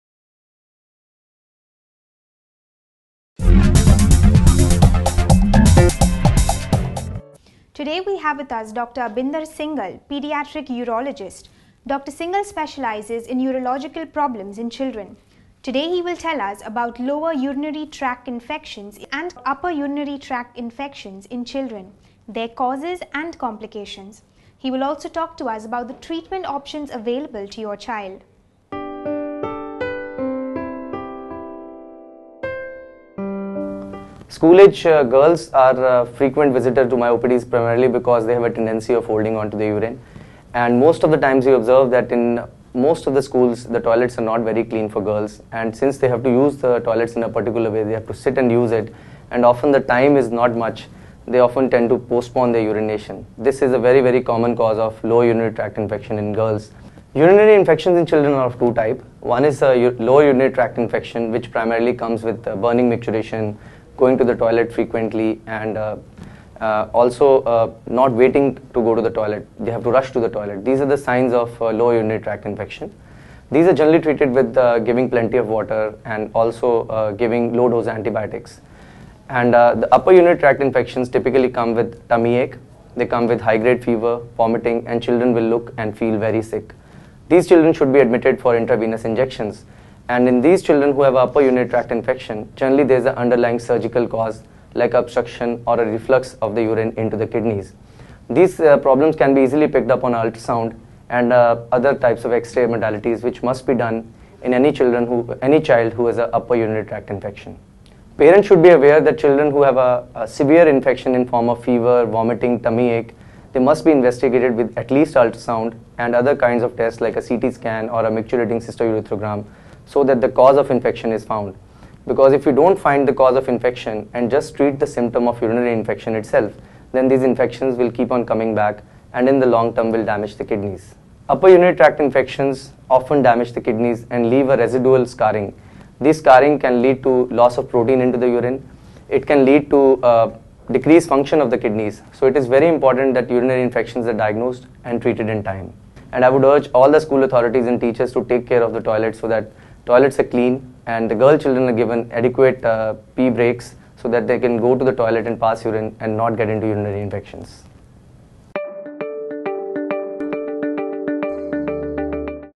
Urinary tract infections in children - Expert opinion
Urinary tract infections in children - Expert opinion Dr Arbinder Singal is a renowned Pediatric Urologist, who specialises in common urological problems found in children. In this video the doctor will tell us about Upper &Lower Urinary tract infections, their treatment and how they can be avoided.